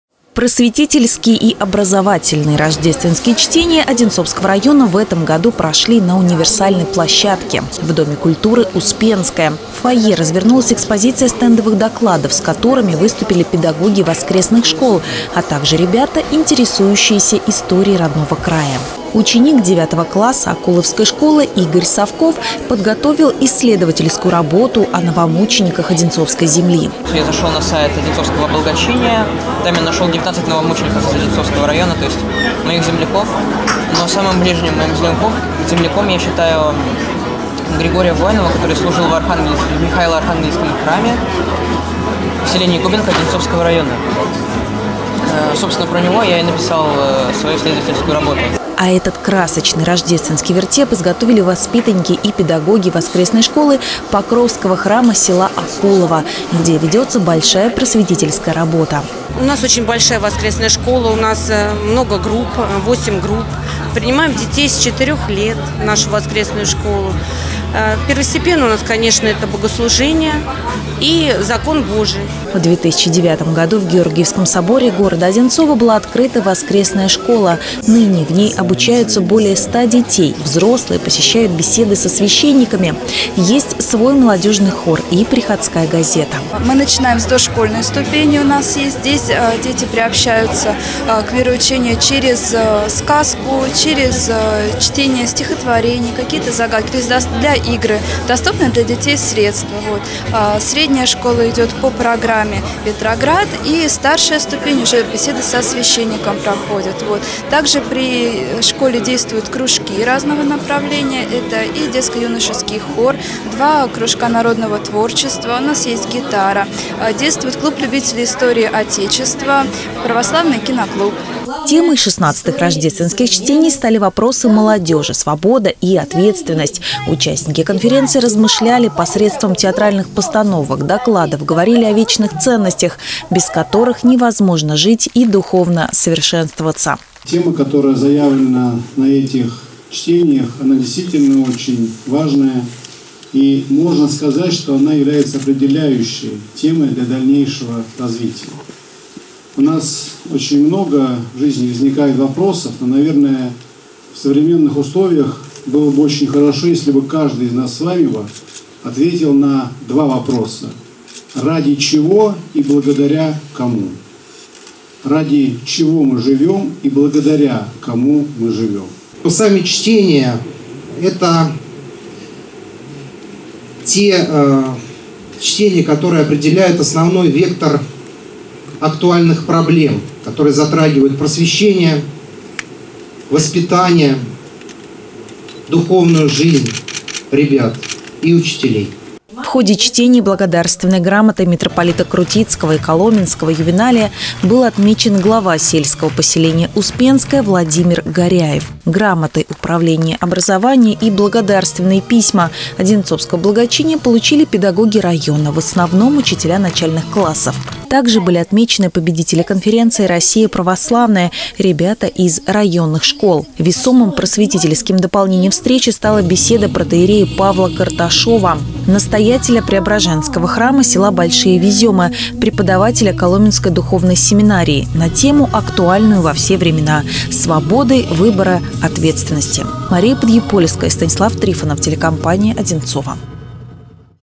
«Рождественские чтения» прошли в Одинцовском районе
Образовательно-педагогический форум стал местом встречи представителей приходов, школ, лицеев, гимназий и дошкольных образовательных учреждений, где ведется работа по православному воспитанию молодежи.